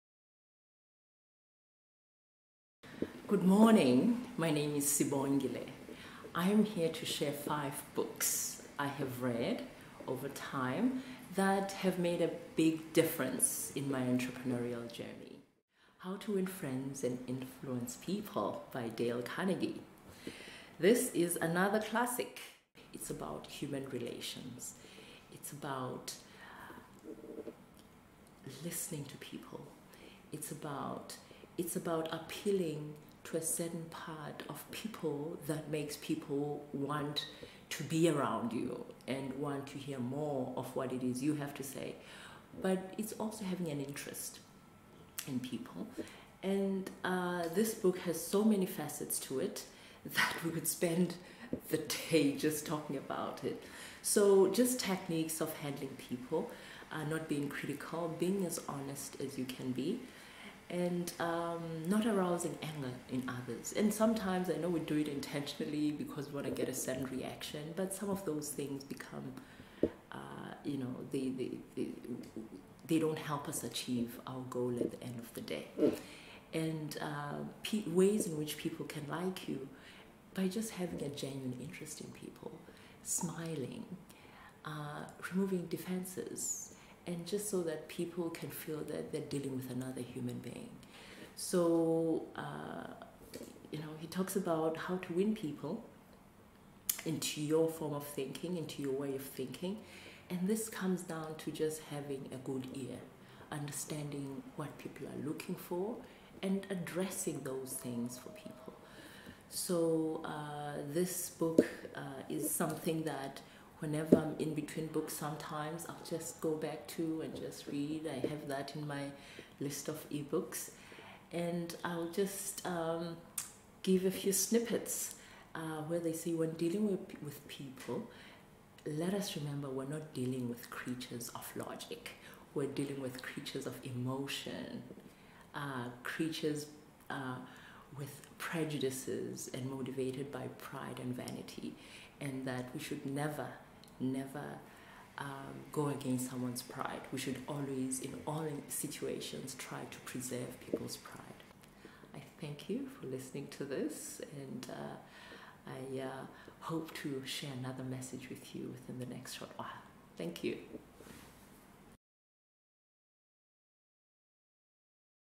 Book 4: ''How To Win Friends And Influence People'' by Dale Carnegie
Sibongile Booi, founder of Second Office shares 5 books that she has read over time that have inspired and made a difference to her entrepreneurial journey.